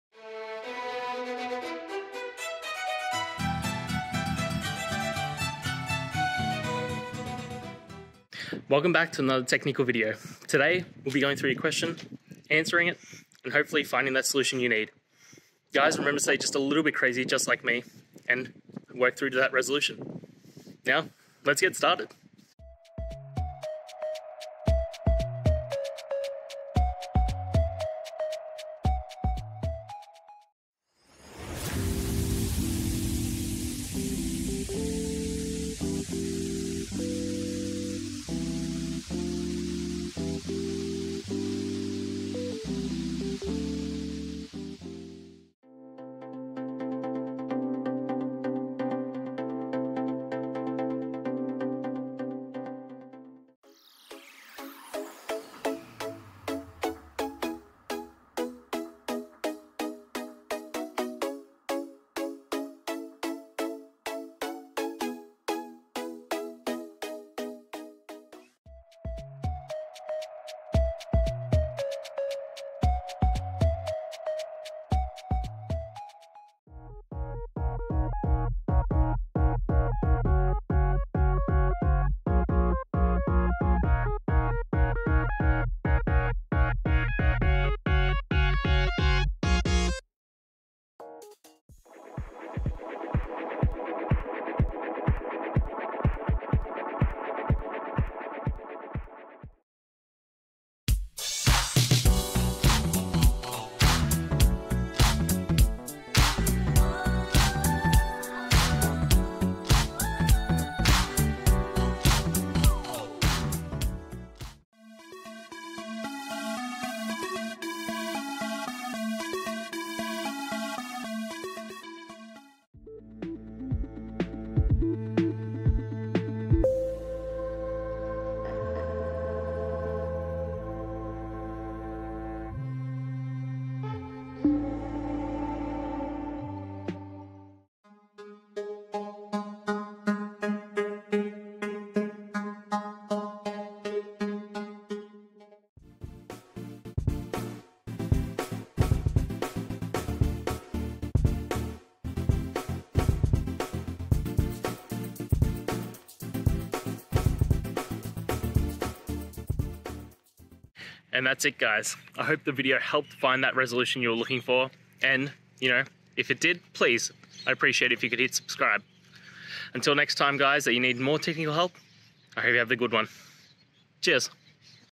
Error:The SDK Build Tools revision (23.0.3) is too low for project ':app'. Minimum required is 2...
android: Error:The SDK Build Tools revision (23.0.3) is too low for project &#39;:app&#39;. Minimum required is 25.0.0

Just wanted to thank those users featured in this video:
Aras (No Longer Available)

Related to: android, android-studio